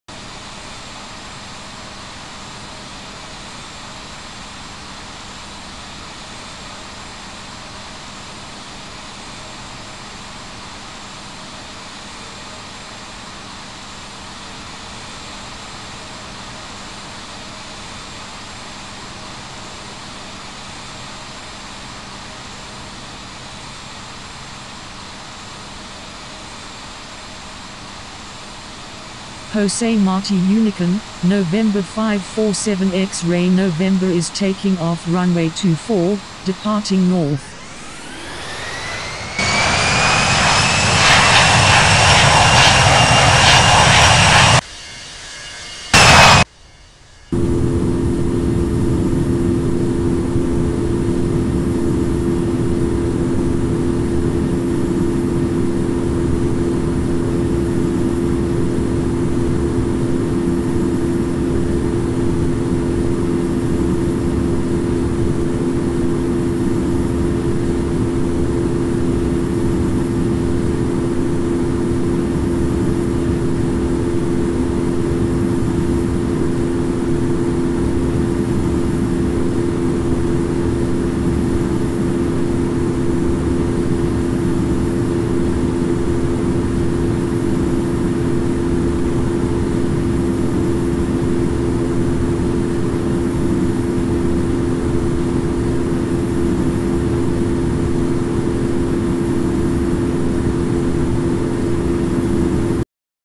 JetBlue E190 Departing Havana José Martí International Airport
JetBlue HAV-JFK (Part 1)